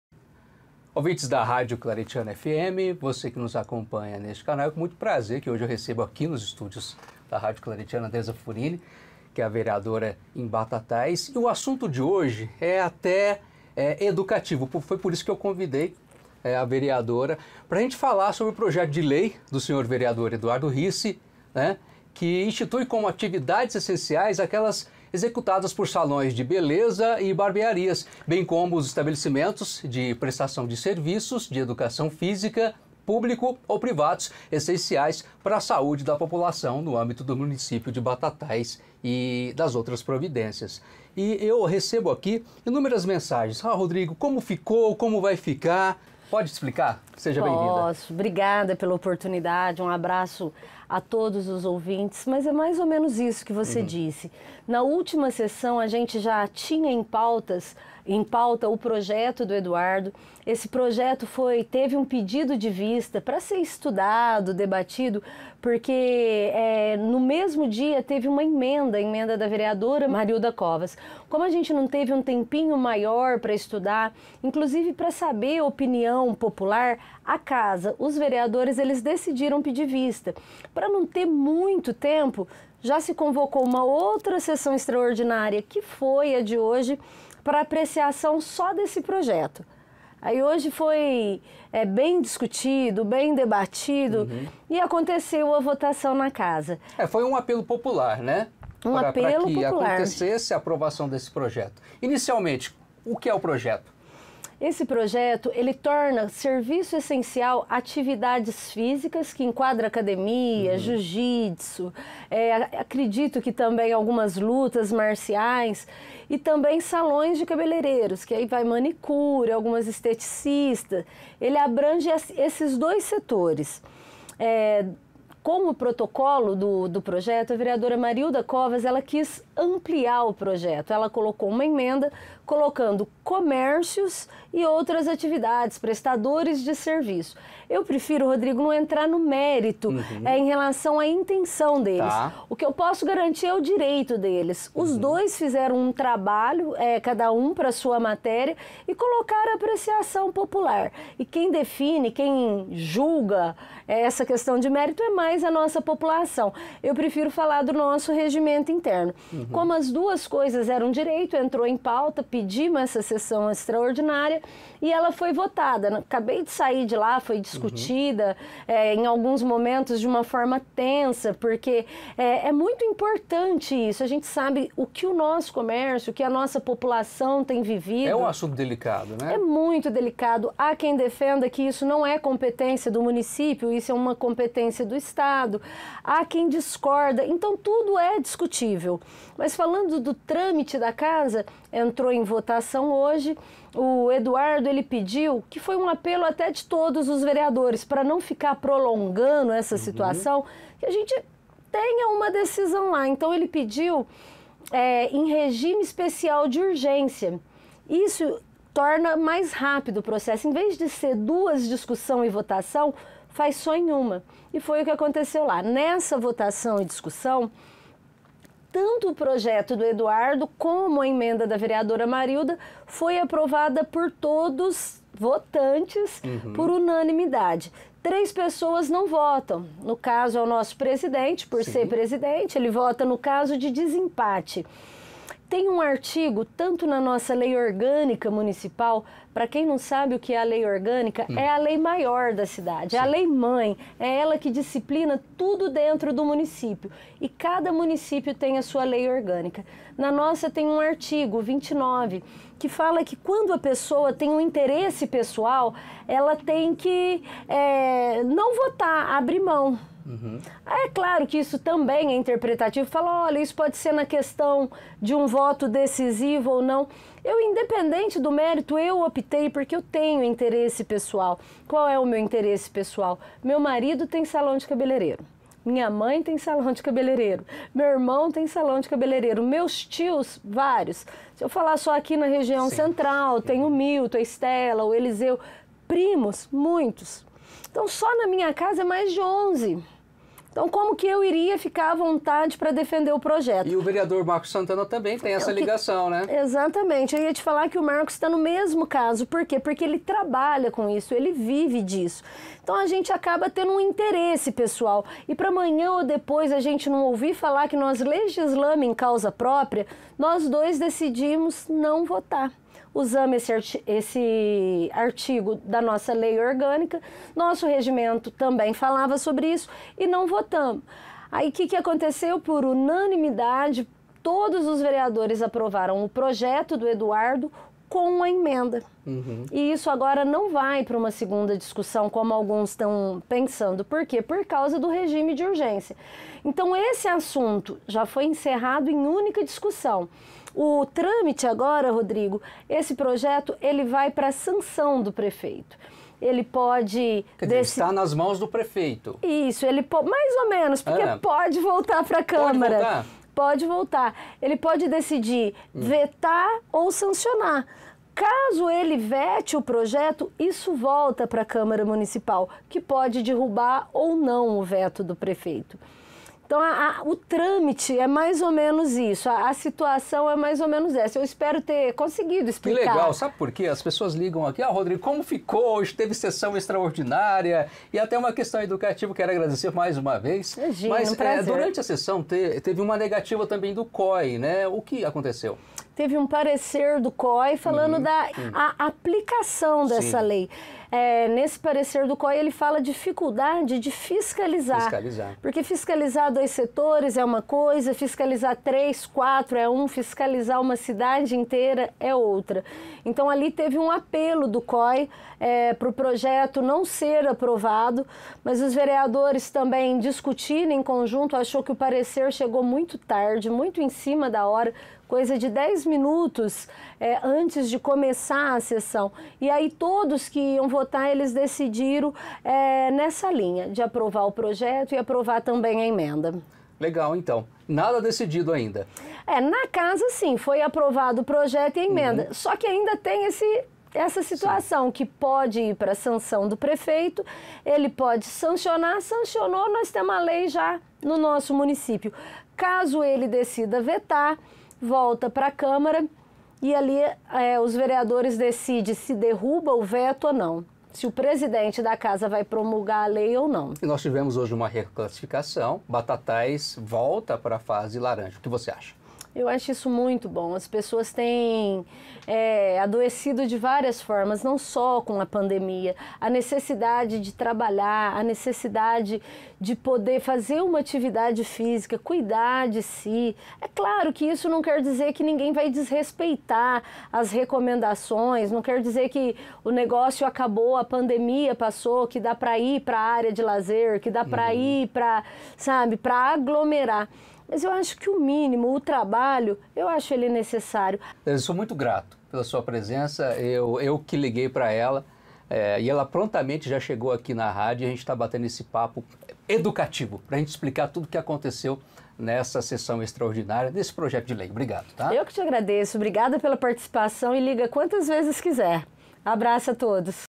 Aprovado projeto de lei que redefine a lista de serviços e setores considerados essenciais! E agora?
A Câmara de Vereadores de Batatais (SP) aprovou nesta Sexta-Feira (5), em sessão extraordinária, um projeto de lei que redefine a lista de serviços e setores considerados essenciais e que, na prática, permite ampliar a flexibilização